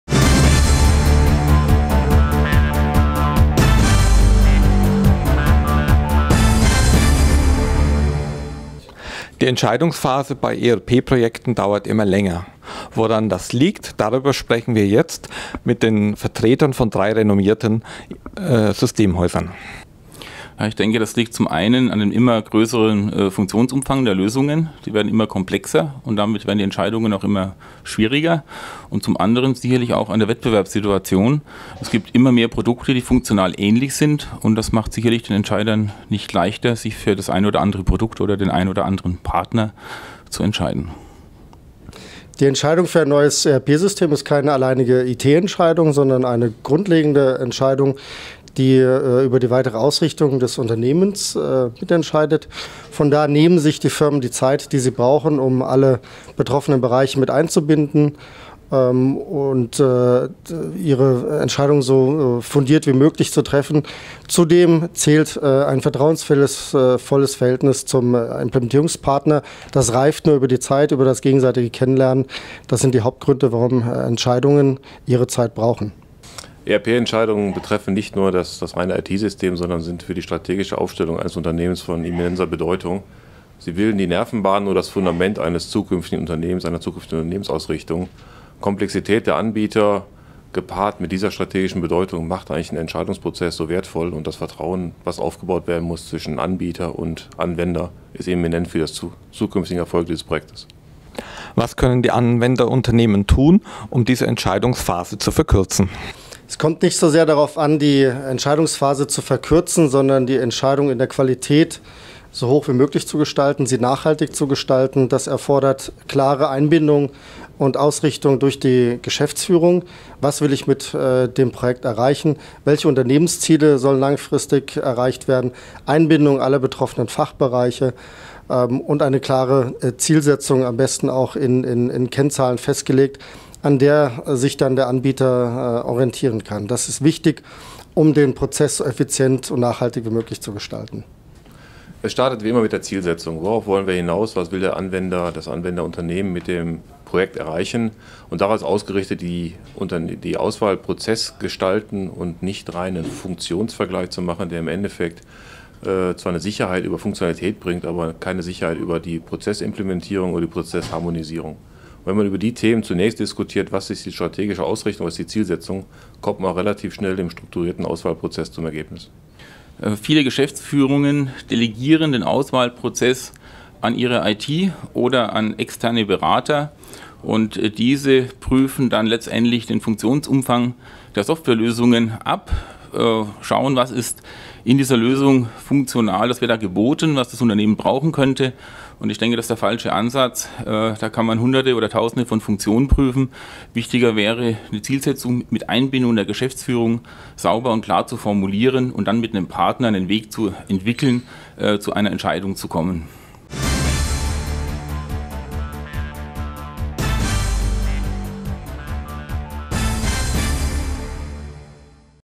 ERP-Projekte: Warum dauert die Entscheidungsphase so lange?
Die Entscheidungsphase bei ERP-Projekten dauert immer länger. Über die Gründe dafür sprach MM-Redakteur Peter Steinmüller mit Ralph Nemnich (Amball Business Software), Dr. Alexander Arnold (Steeb Anwendungssysteme) und Dr. Andreas Pauls (Itelligence).